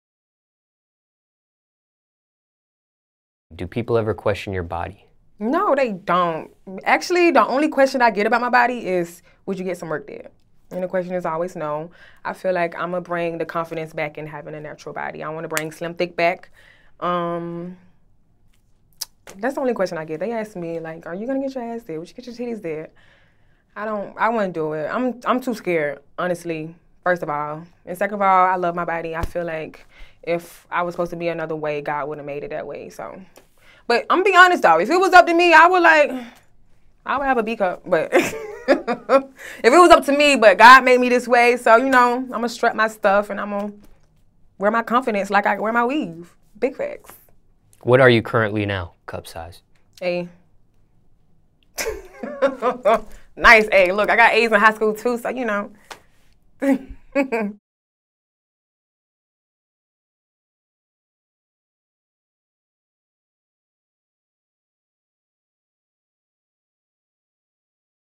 Is Pap Chanel Body Natural? She Gets Honest Here (part 3)
Millidgeville, GA born/raised/Atlanta residing recording artist, Pap Chanel, sits down with DJ Smallz and reveals her sexuality.

Did you guess right or wrong before watching this clip?